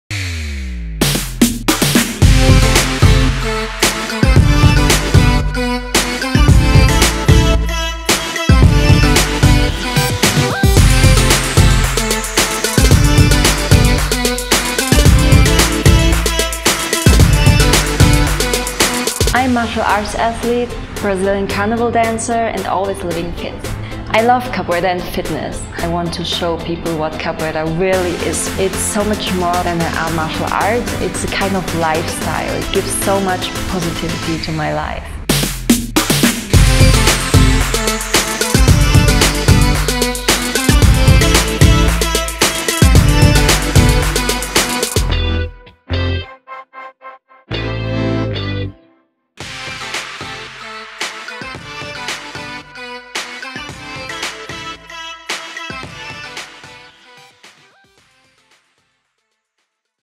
Desirée Mitulla | Martial Artist, Carnival Dancer & OnlyFans Creator
Check out those moves! Martial artist and Brazilian carnival dancer Desirée Mitulla is spin-kicking her way onto OnlyFans! 🤸‍♀️

Desirée will be sharing exclusive Capoeira tutorials, training footage, and even teaching languages too, she speaks 5! 🇬🇧🇧🇷🇩🇪🇲🇽🇫🇷

So step into the ring and start the conversation at: patativa